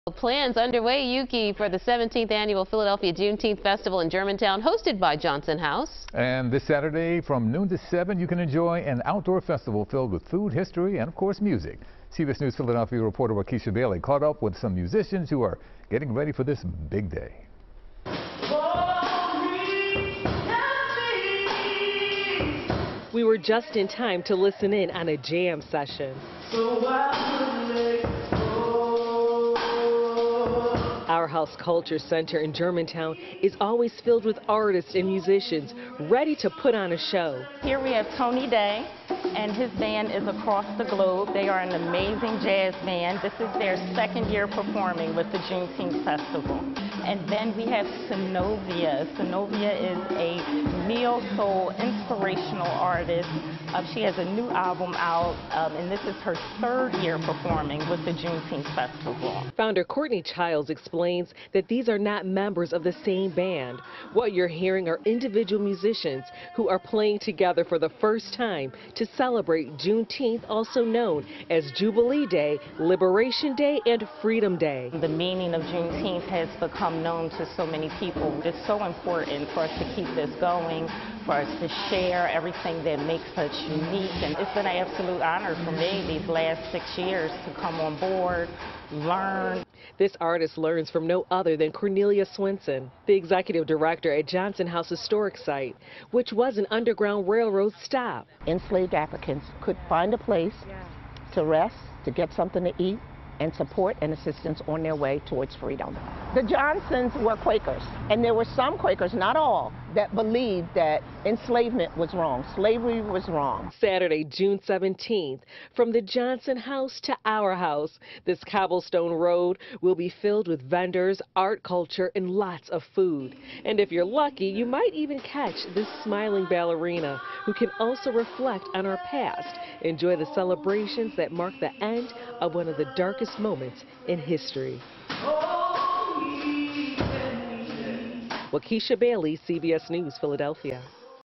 Juneteenth Festival in Germantown to highlight music and culture
Wakisha Bailey caught up with some musicians getting ready for the event.